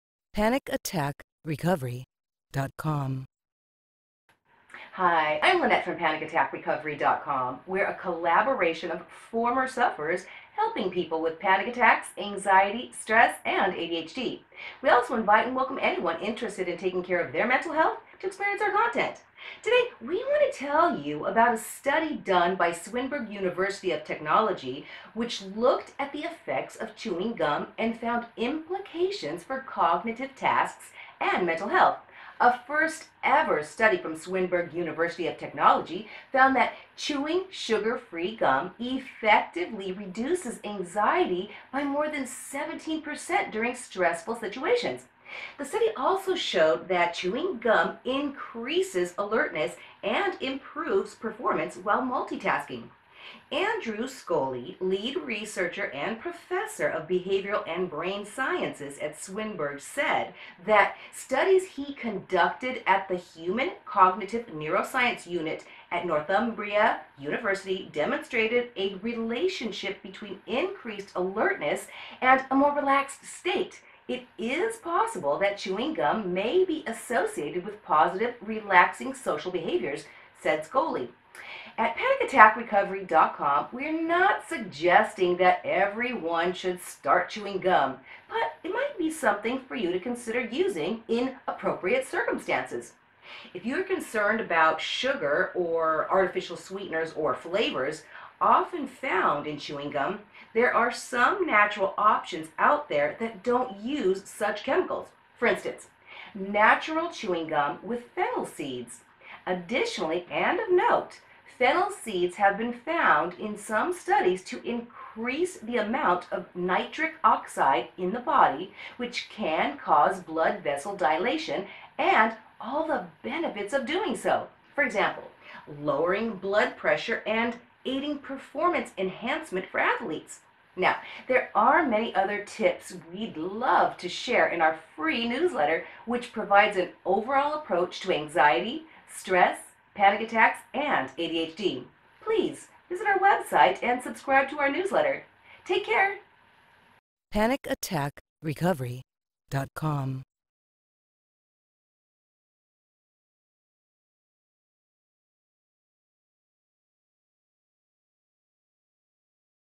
Weekly ADHD and Anxiety Tips: Chewing Gum, Cognitive Benefits & Mental Health
In this video, we want to tell you about a study done by Swinburne University of Technology, which looked at the effects of chewing gum and found implications for cognitive tasks and mental health.